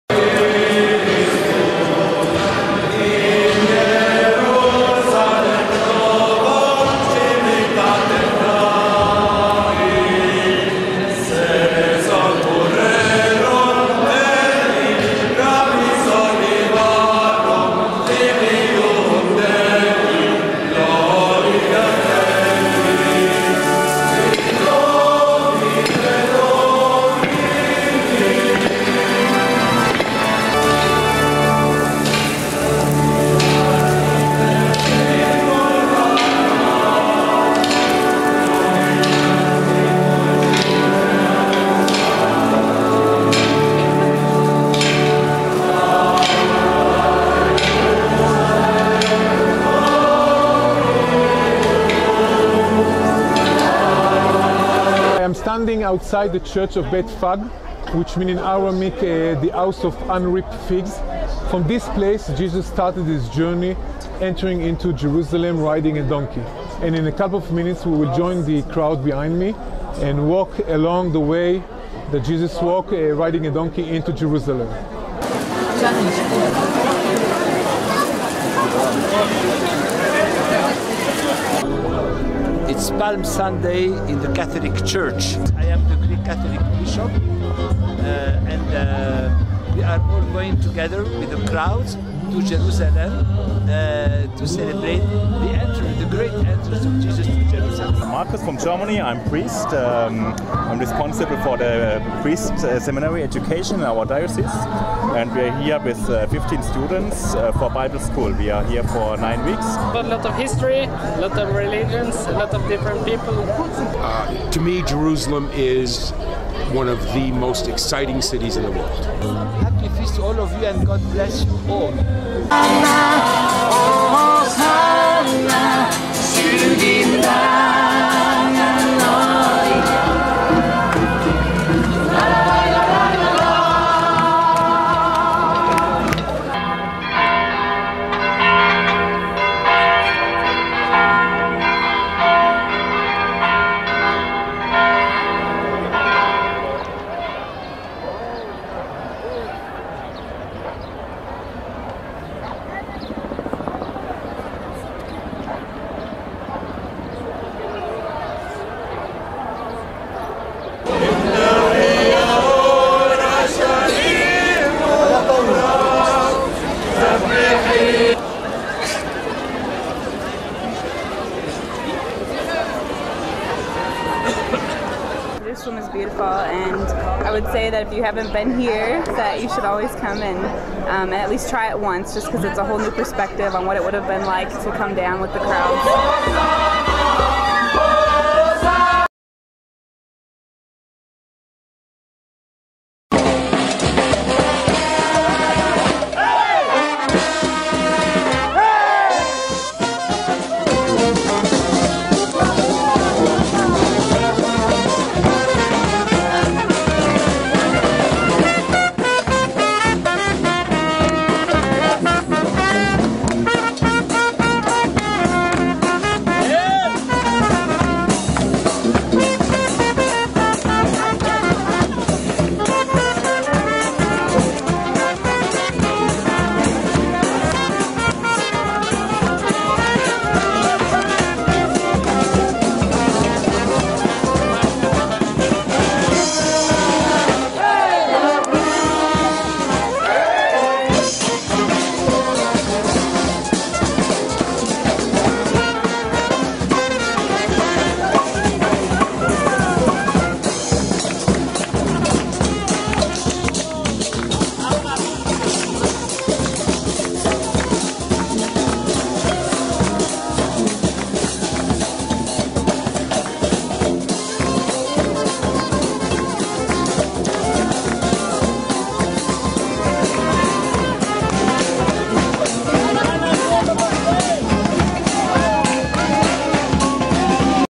Palm Sunday Jerusalem!! 2013 HD
First part from jerusalemexperience.com and second in tel aviv last year